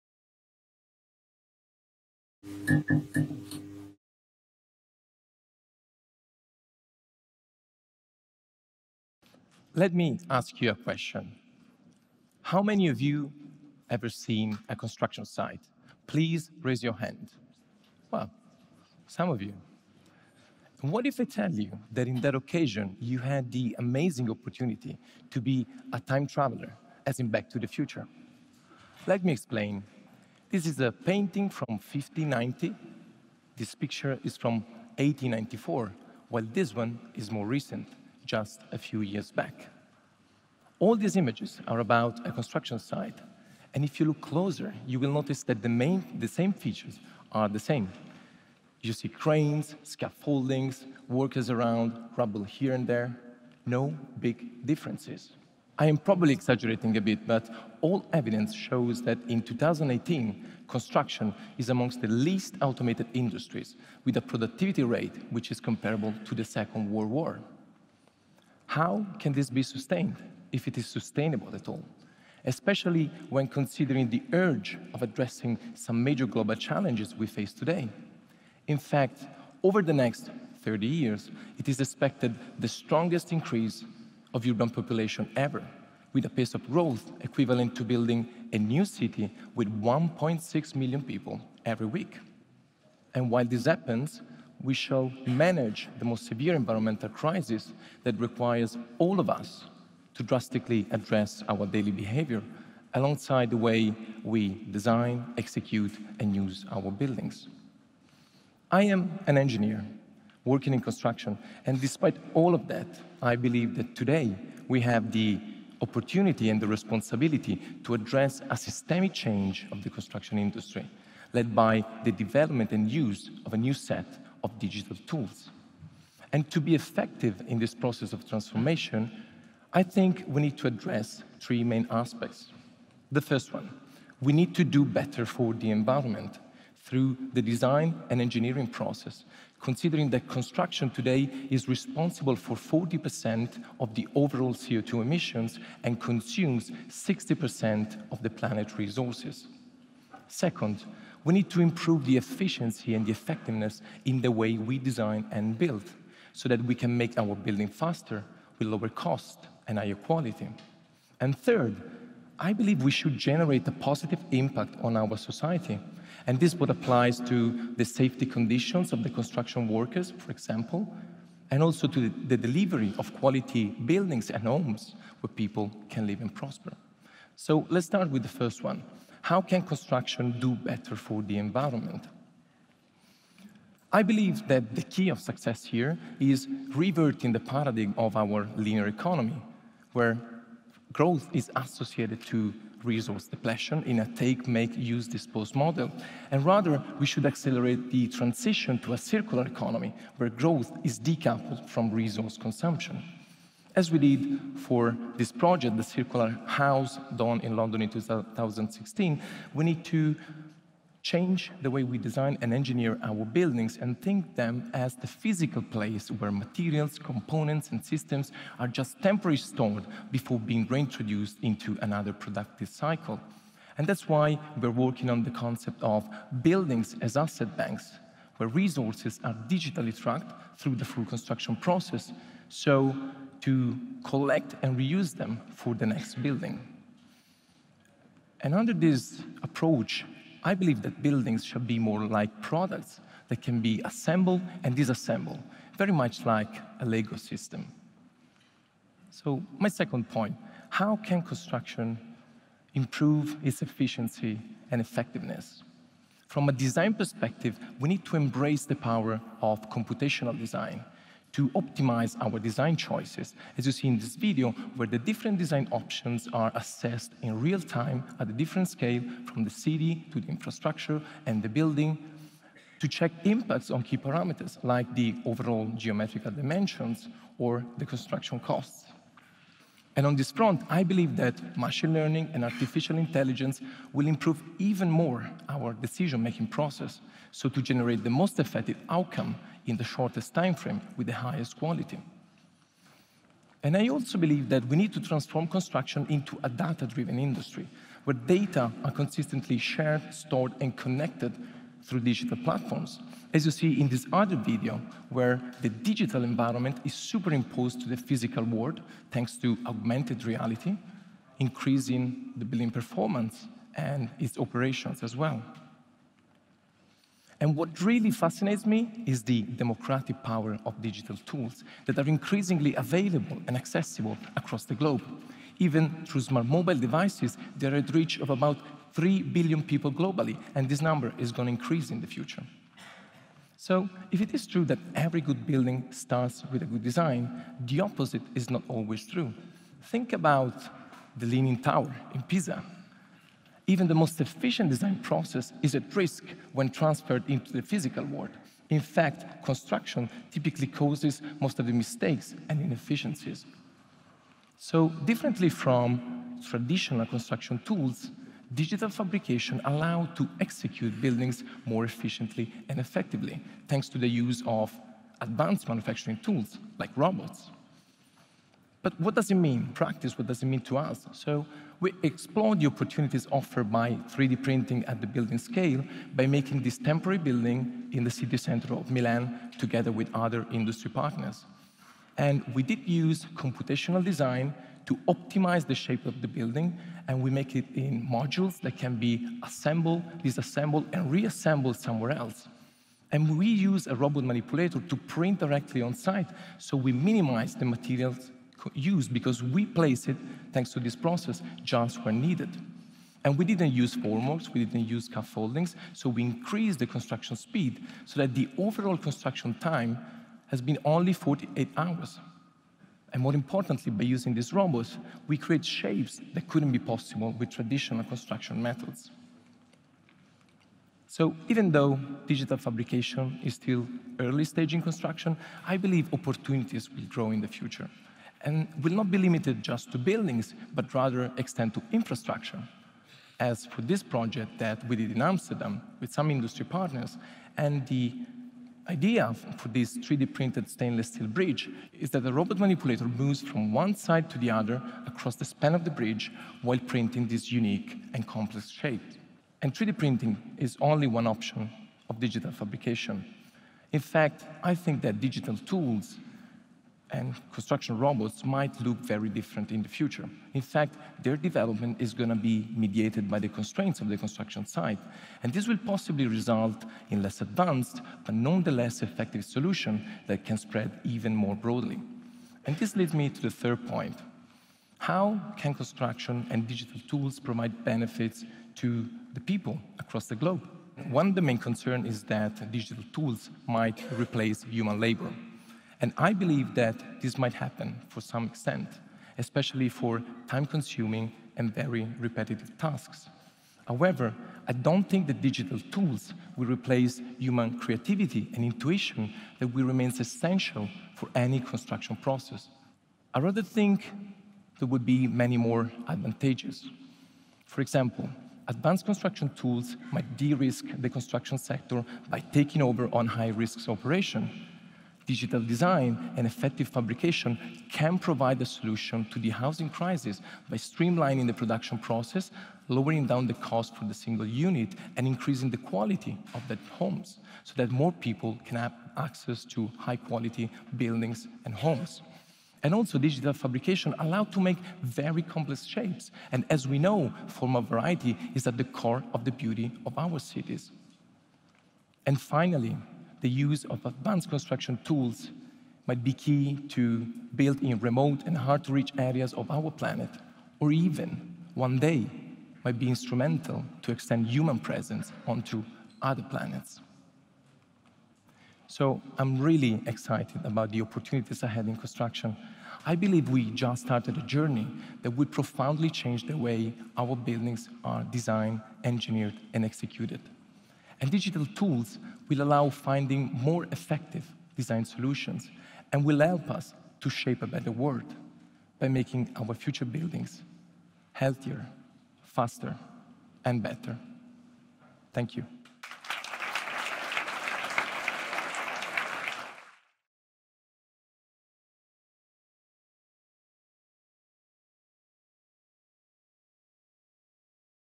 Can we make better buildings? | Guglielmo Carra | TEDxTorino
Guglielmo Carra talks about the opportunities offered by robots and process automation to reduce the inefficiencies in construction and make future buildings better, more efficiently and sustainably. Ingegnere, progettista e scienziato dei materiali, Guglielmo si dedica a livello internazionale allo sviluppo di nuove tecnologie applicate a diversi settori delle costruzioni e alla gestione di processi di innovazione e trasferimento tecnologico.
Guglielmo ha lavorato e vissuto negli Stati Uniti, nel Regno Unito e in Germania ed è attualmente in Italia. È un alumno dell’Università di Pisa, dove si è laureato, del Politecnico di Milano, dove ha ricevuto il suo Dottorato di ricerca, e del Massachusetts Institute of Technology (MIT) dove è stato Visiting Student e Scholar.
Dal 2012 Guglielmo lavora per Arup – società leader mondiale per la progettazione applicata al settore delle costruzioni. Qui Guglielmo è alla guida del team Europeo di Consulenza dei Materiali e, per il Gruppo Italiano, è responsabile dello sviluppo e della strategia di Trasformazione Digitale. This talk was given at a TEDx event using the TED conference format but independently organized by a local community.